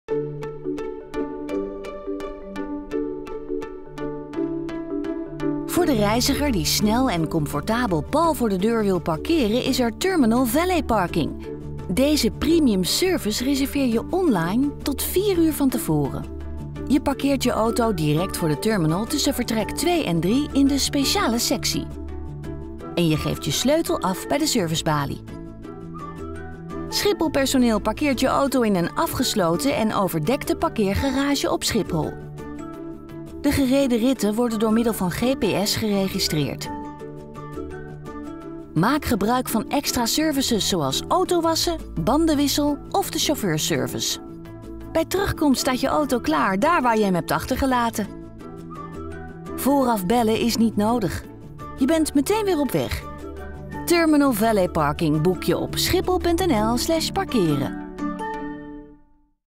Luxe parkeerservice van Schiphol bij de terminal
Voor de reiziger die snel en comfortabel pal voor de deur wil parkeren, is er Terminal Valet Parking. Je parkeert je auto direct voor de terminal tussen vertrekhal 2 en 3 en je geeft je sleutel af bij de servicebalie. Wij parkeren je auto in een afgesloten en overdekte parkeergarage op Schiphol. Ook kun je deze parkeerservice uitbreiden met extra’s, zoals autowassen, bandenwissel of de chauffeursservice.